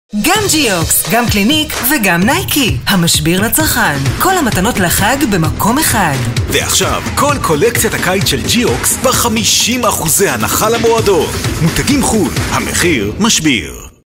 50% sale geox shoes
חג שמח מהמשביר!
במיוחד לכבוד החג קבלו 50% הנחה על כל קולקציית הקייץ של נעלי גאוקס! לחברי מועדון בלבד.